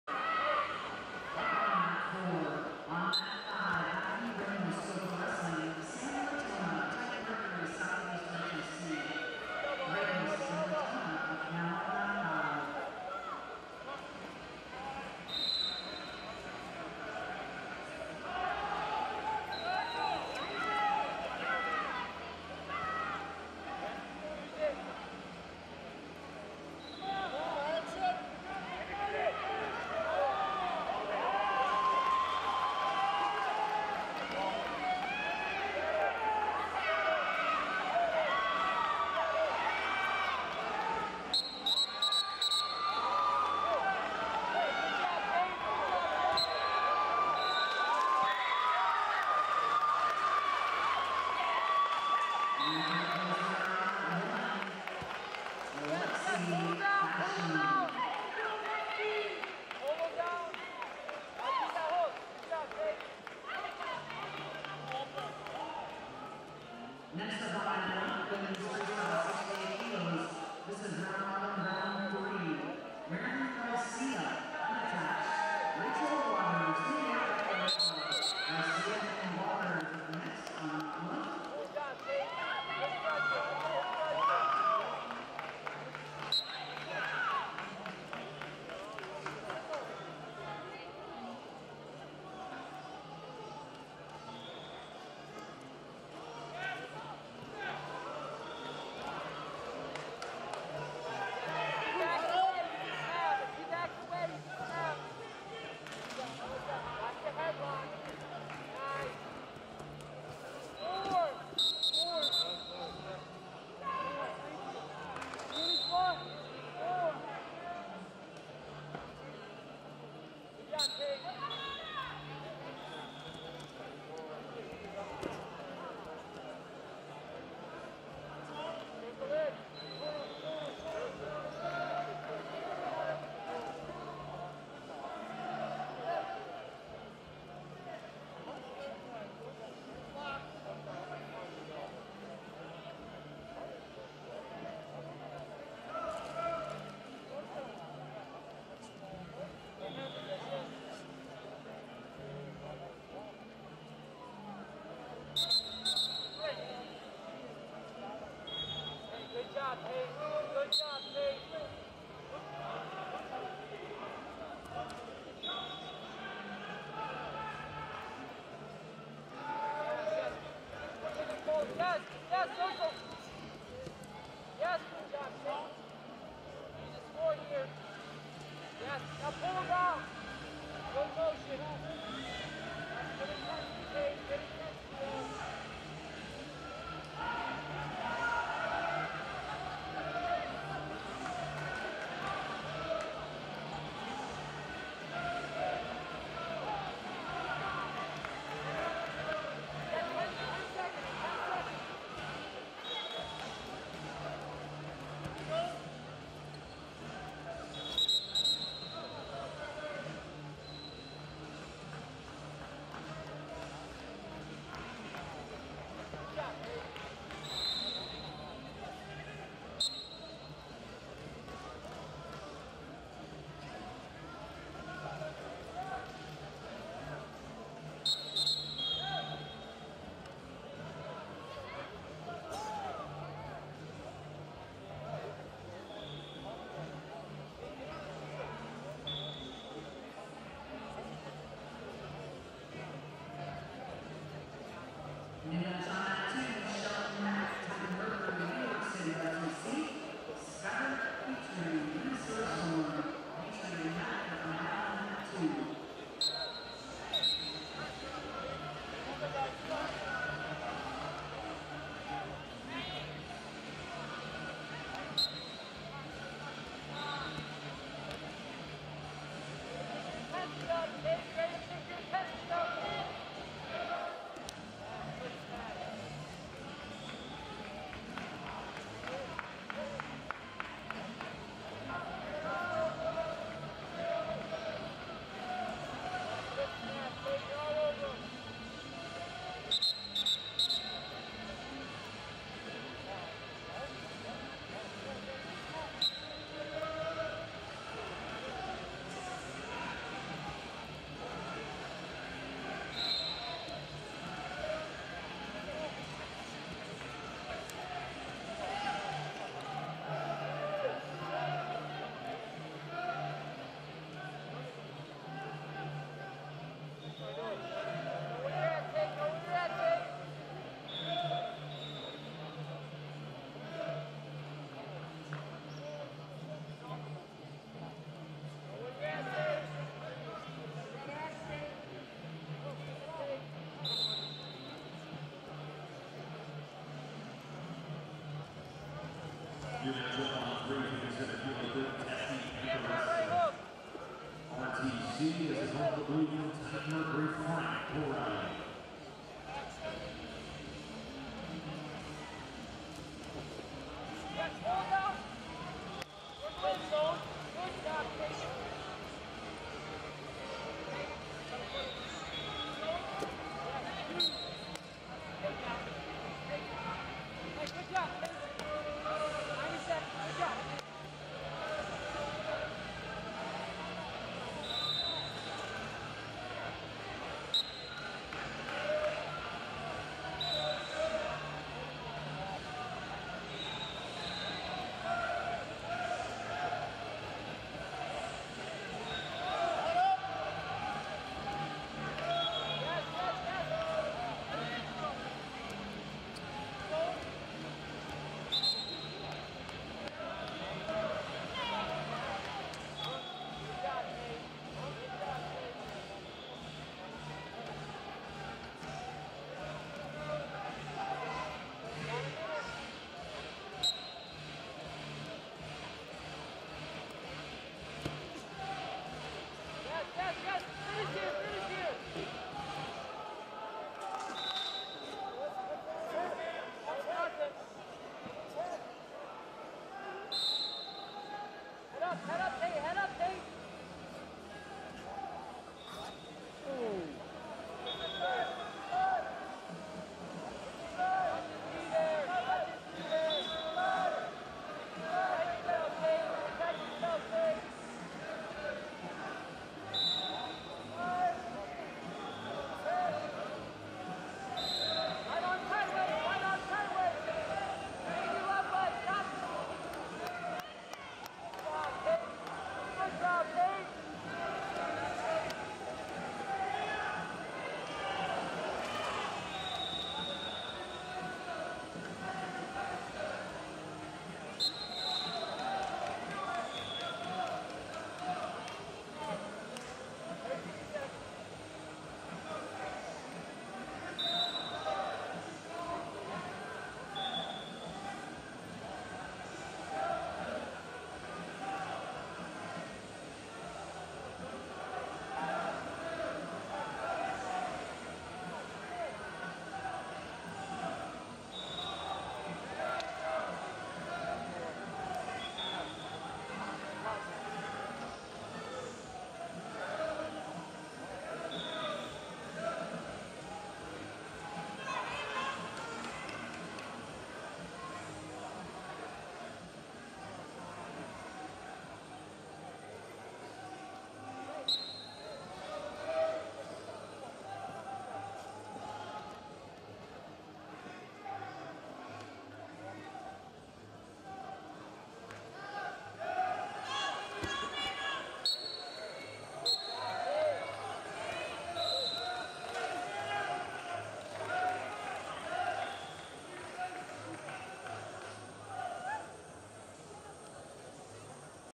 125 Kg Consolation David Tate Orndorff TMWC OHIO RTC Vs Aj Nevills TMWC Valley RTC
2020 Senior Nationals - 125 Kg Consolation David Tate Orndorff TMWC OHIO RTC Vs Aj Nevills TMWC Valley RTC